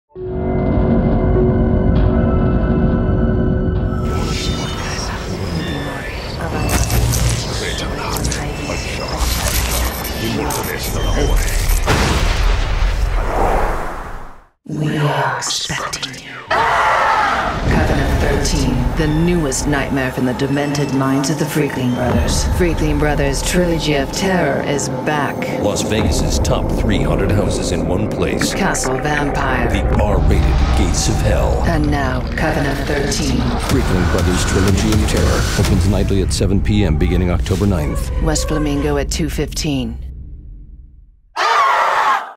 Freakling Bros. Trilogy of Terror
Quiet Film is proud to have been select to create The Geary Company's ad for Freakling Bros. Trilogy of Terror! Happy Halloween!